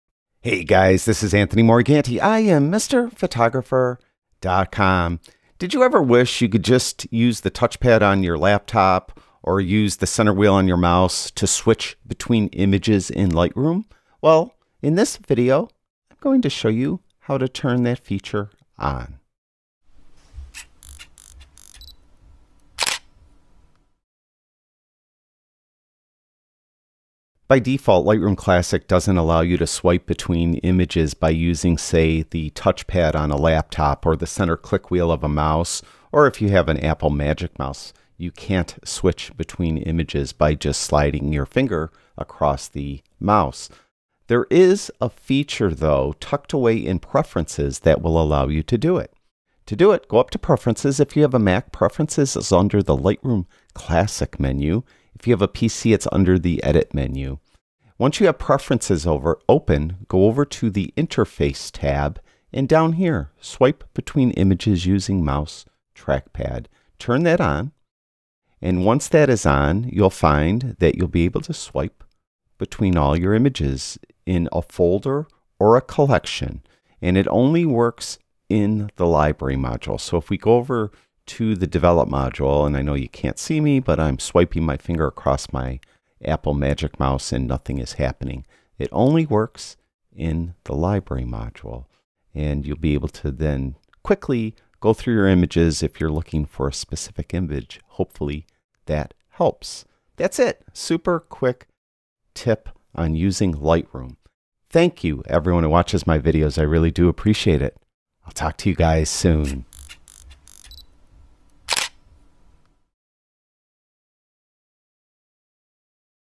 HIDDEN Lightroom Feature - SWIPE Between Images
In this video I show you a setting that is tucked away in the preferences menu that will allow you to swipe on a touchpad or use the center mouse wheel to move between images.

The images shown in the video are from a photo project I did. You can see it here:

Checkout my Recommended Gear List:

Thumbnail Photo by Nik MacMillan on Unsplash